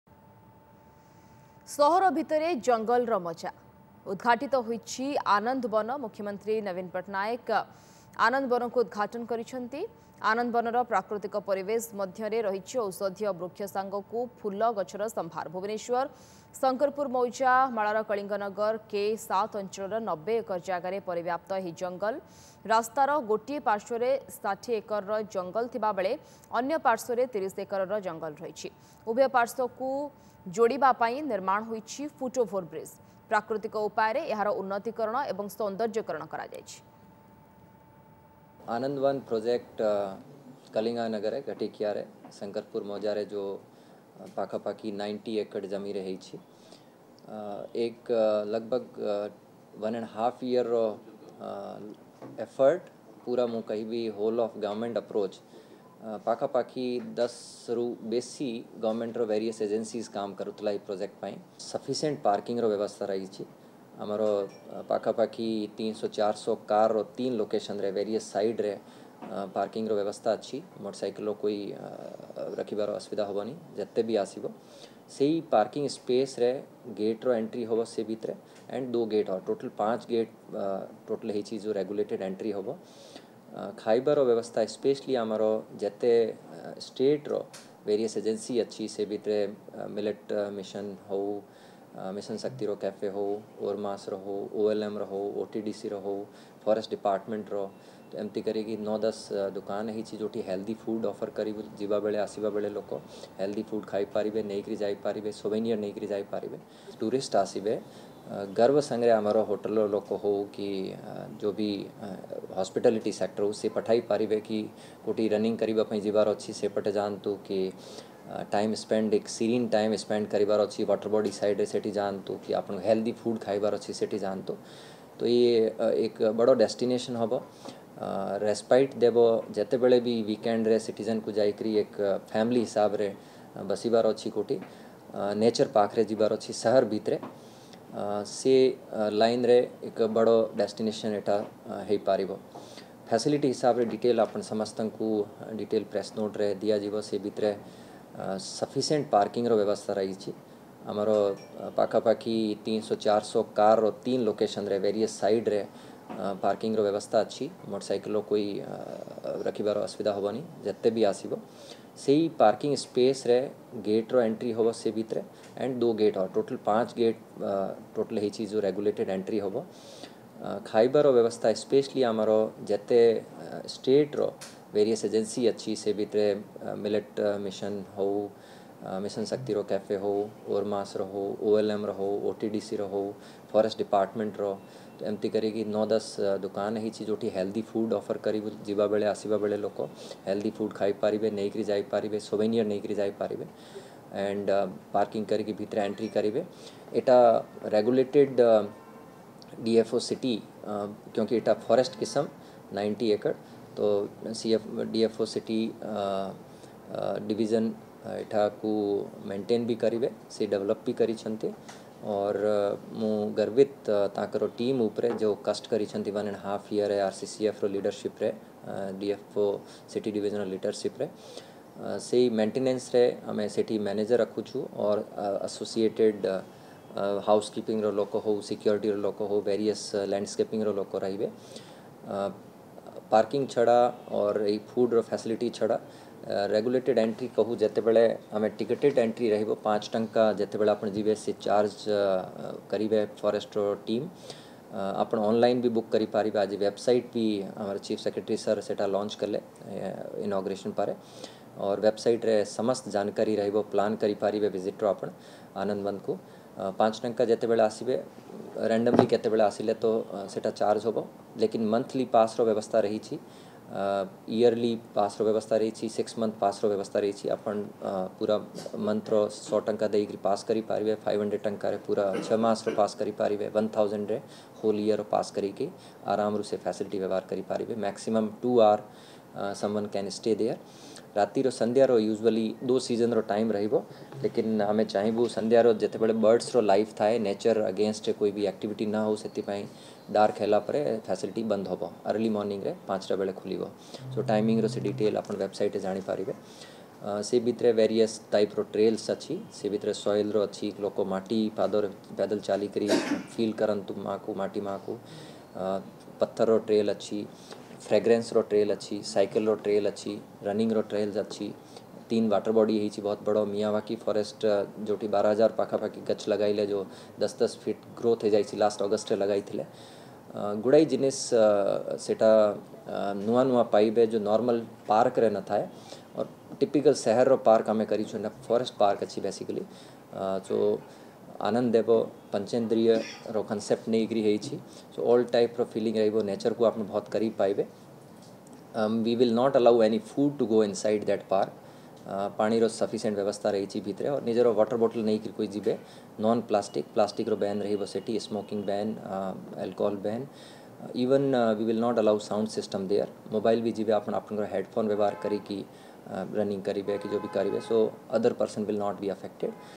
90-acre urban forest- ‘Anandavan’ inaugurated in Bhubaneswar
90-acre urban forest- ‘Anandavan’ inaugurated in Bhubaneswar

ସହର ଭିତରେ ଜଙ୍ଗଲର ମଜା । ଉଦଘାଟିତ ହୋଇଛି ଆନନ୍ଦ ବନ । ମୁଖ୍ୟମନ୍ତ୍ରୀ ନବୀନ ପଟ୍ଟନାୟକ ଆନନ୍ଦବନକୁ ଉଦଘାଟନ କରିଛନ୍ତି । ଆନନ୍ଦବନର ପ୍ରାକୃତିକ ପରିବେଶ ମଧ୍ୟରେ ରହିଛି ଔଷଧୀୟ ବୃକ୍ଷ ସାଙ୍ଗକୁ ଫୁଲ ଗଛର ସମ୍ଭାର।